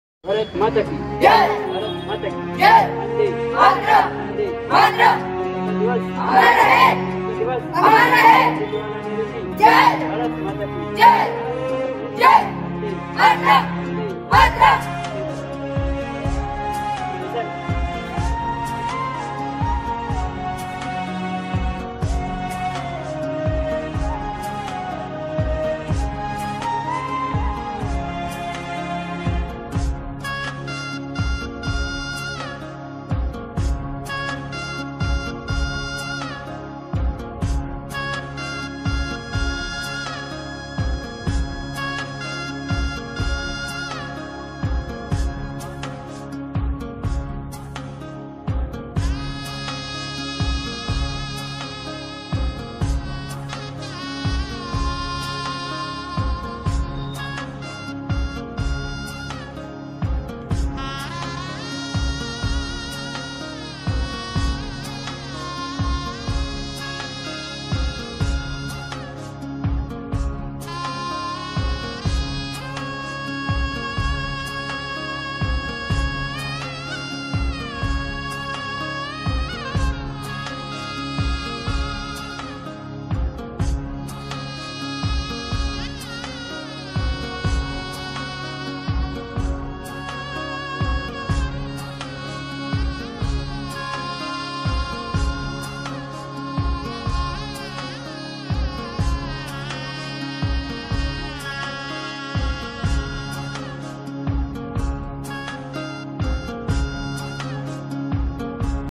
Flage Down!! 15 अगस्त!! 2024, Sages Chandrapur 🇮🇳🙏❤️🔥 Video.
देखिए अगर kuch गलती ho तो माफ कीजिएगा । लेकिन जीतना व्यवस्था बना उतना से कोशिश किया हूं। की सब badiya ho aur noyam ka ulanghan ना  ho।।  Hamare school के tiranga jhanda down kiya गया।।